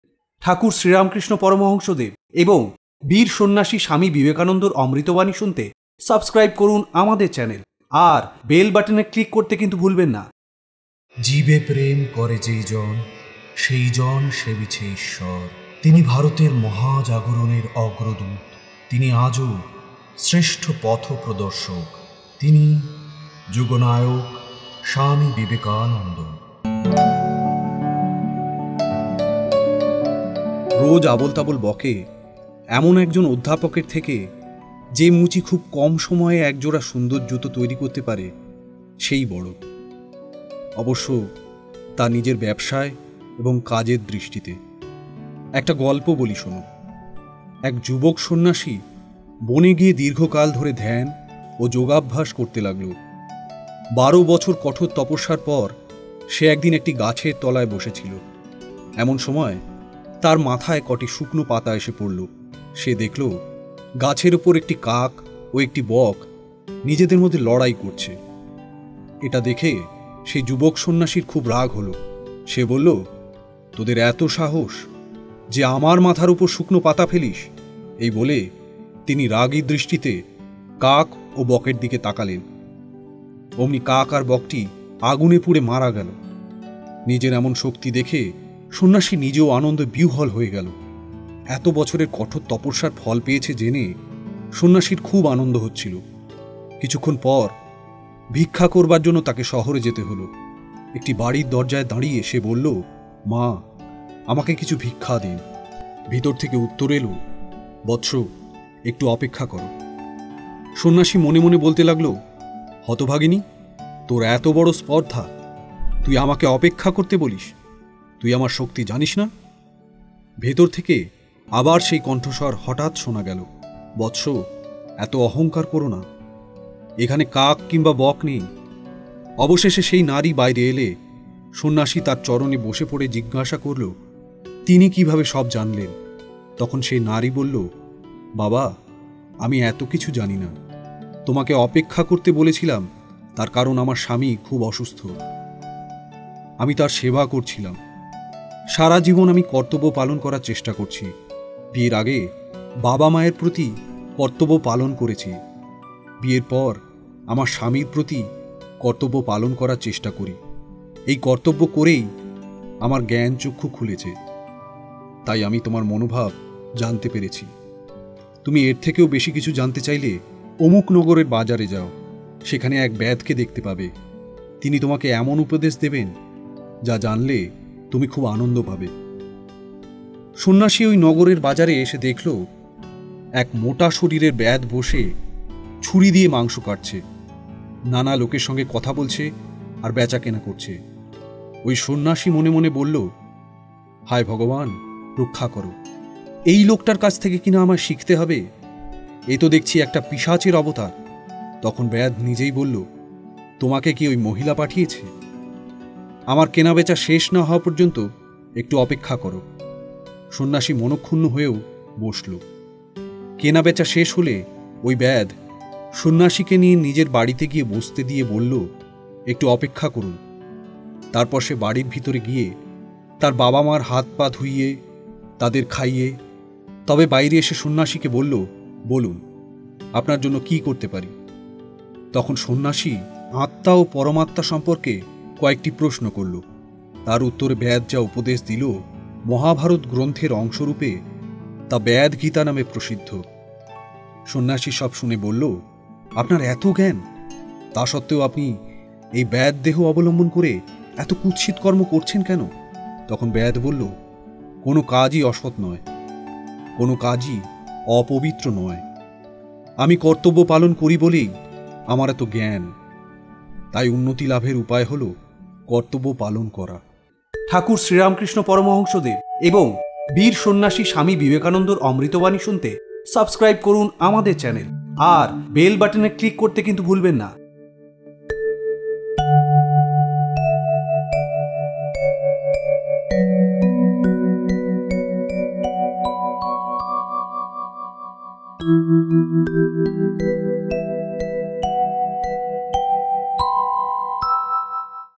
কাজের দৃষ্টিতে যে বড়, মানুষ হিসেবেও সেই বড় | Kathamrita | Vivekananda
A cobbler is more useful than a professor who speaks meaninglessly. A cobbler who can make a shoe within lesser time.  Who is doing his or her duty within time than the man who is doing nothing but has vast knowledge.